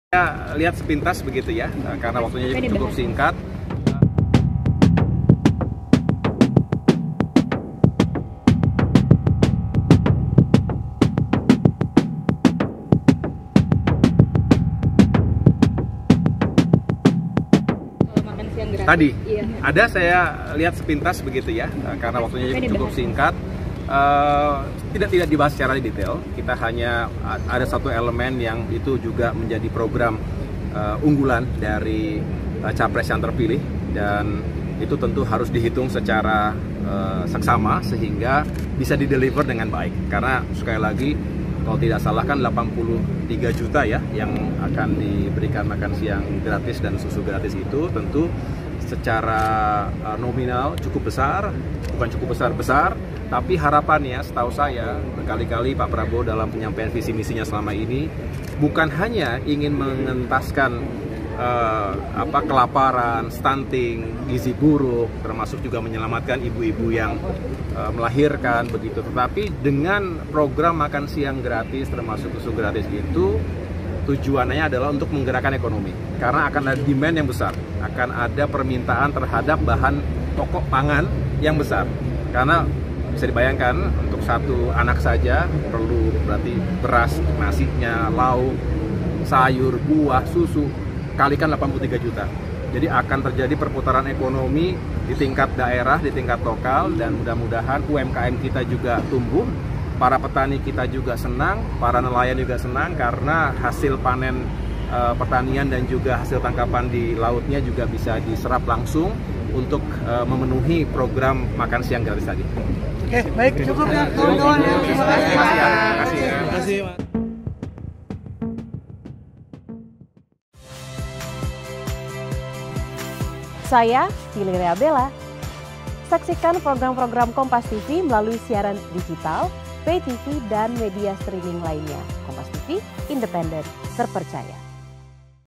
AHY Bocorkan Program Makan Siang Gratis di Sidang Kabinet Bersama Jokowi
KOMPAS.TV - Menteri Agraria dan Tata Ruang/Kepala Badan Pertanahan Nasional (ATR/BPN), Agus Harimurti Yudhoyono (AHY), mengungkapkan bahwa program makan siang gratis sempat menjadi topik pembahasan dalam rapat kabinet paripurna yang diadakan pada hari Senin, 26 Februari 2024.

Meskipun menjadi salah satu agenda, program yang diusung oleh pasangan calon presiden dan wakil presiden nomor urut 02, Prabowo Subianto dan Gibran Rakabuming Raka, tersebut tidak dibahas secara mendalam.

Menurut AHY, keterbatasan waktu menjadi alasan utama mengapa program tersebut tidak dapat dibahas secara detail.

"Ada saya lihat sepintas begitu ya, tapi karena waktu terlalu singkat tidak dibahas secara detail," ujar AHY saat ditemui di Istana Negara.

Program makan siang gratis ini merupakan salah satu dari berbagai program yang dijanjikan oleh Prabowo-Gibran dalam kampanye mereka.

Meskipun belum dibahas secara luas dalam rapat kabinet, program ini tetap menarik perhatian publik dan diharapkan dapat segera mendapatkan sorotan lebih lanjut dalam waktu dekat.